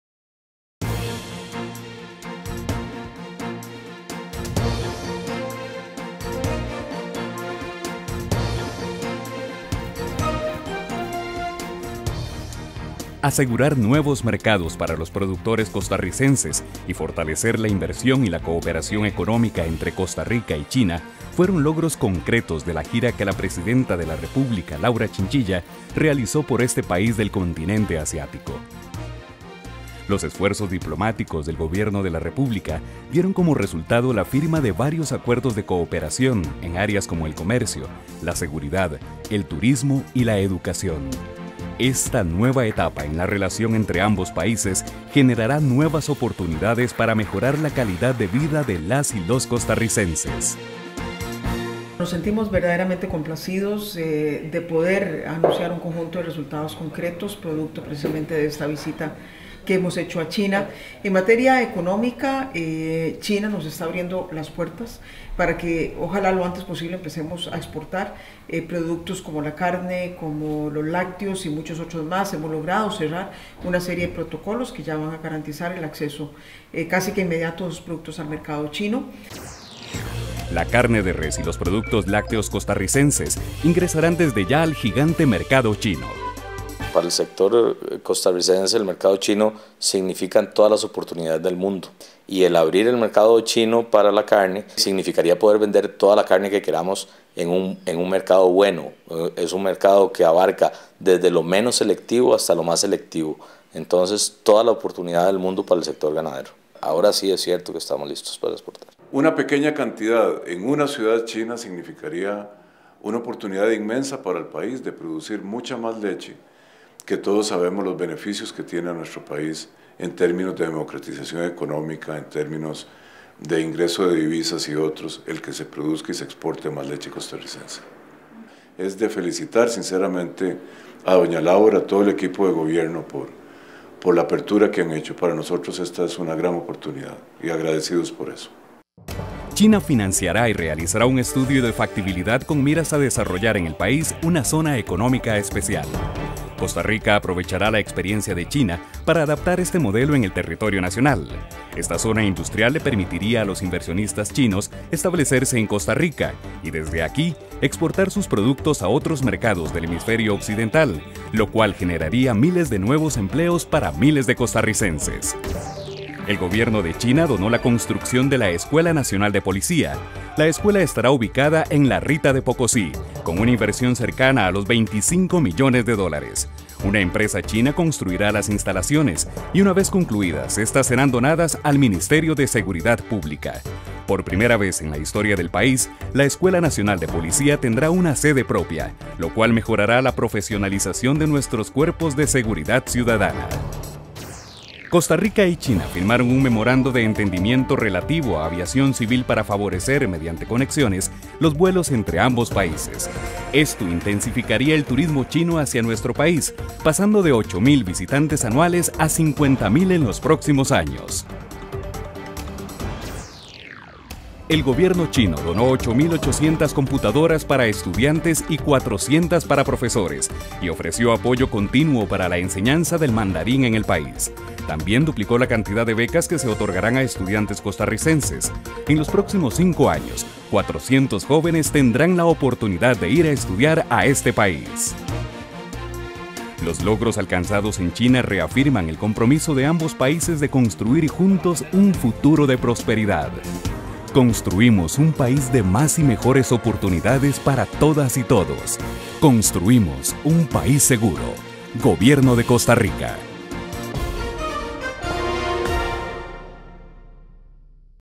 Cadena Nacional, 26 de agosto del 2012
Asegurar nuevos mercados para los productores costarricenses, fortalecer la inversión y la cooperación económica entre Costa Rica y China, fueron logros concretos de la gira que la Presidenta de la República Laura Chinchilla realizó por este país del continente asiático. Los esfuerzos diplomáticos del Gobierno de la República dieron como resultado la firma de varios acuerdos de cooperación en áreas como el comercio, la seguridad, el turismo y la educación.

Esta nueva etapa en la relación entre ambos paises,generan nuevas oportunidades y mejoran la calidad de vida de los costarricenses.

La carne de res y los productos lácteos costarricenses, ingresarán desde ya al gigante mercado chino.

China financiará y realizará un estudio de factibilidad para desarrollar en el país una zona económica especial. Costa Rica aprovechará la experiencia de China, para adaptar este modelo en el territorio nacional, esta zona industrial le permitiría a los inversionistas chinos establecerse en Costa Rica y desde aquí exportar sus productos a otros mercados del hemisferio occidental, lo cual generaría miles de nuevos empleos para miles de costarricenses.

El Gobierno de China donó la construcción de la Escuela Nacional de Policía, la escuela estará ubicada en La Rita de Pococí con una inversión cercana a los $25 millones.

Una empresa china construirá las instalaciones, y una vez concluídas, serán donadas al Ministerio de Seguridad Pública. Por primera vez en la historia del país, la Escuela Nacional de Policía tendrá una sede propia, lo cual mejorará la profesionalización de nuestros cuerpos de seguridad ciudadana.

Costa Rica y China firmaron un memorando de entendimiento relativo a Aviación Civil para fortalecer las conexiones entre ambos países, esto intensificaría el turismo chino hacia nuestro país, pasando de 8 mil visitantes anuales a 50 mil en los próximos años.

El gobierno chino donó 8800 computadoras para estudiantes y 400 para profesores, y ofreció apoyo al INA para la enseñanza de mandarín en el país. También duplicó la cantidad de becas que se otorgarán a los estudiantes costarricenses, en los próximos 5 años, 400 jóvenes tendrán la oportunidad de ir a estudiar a este país.
Los logros alcanzados en China reafirman el compromiso de ambos países de construir juntos un futuro de prosperidad. Construimos un país de más y mejores oportunidades para todas y todos.